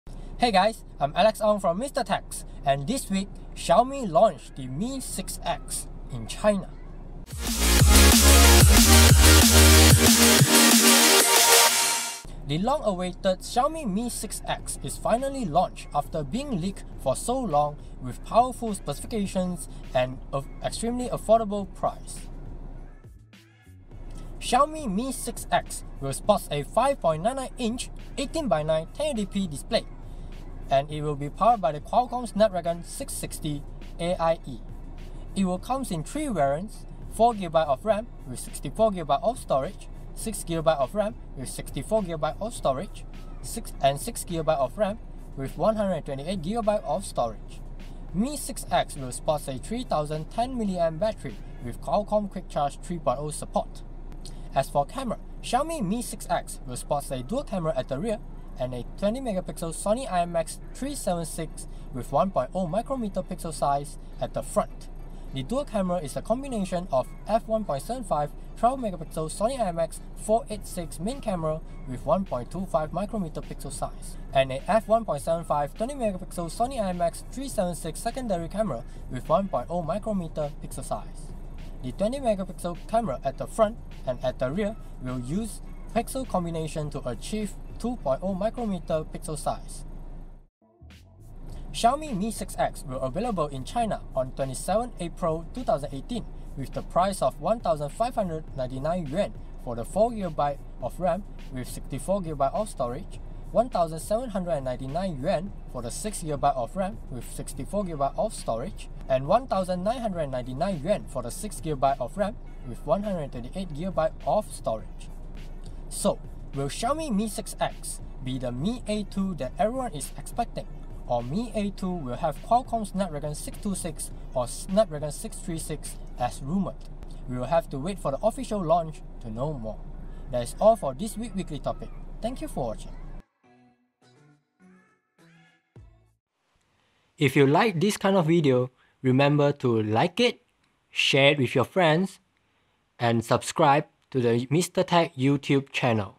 Weekly Topic 065: Xiaomi Mi 6X is Launched in China | Mister Techs
The long awaited Xiaomi Mi 6X is finally launched after being leaked for so long with powerful specifications and extremely affordable price.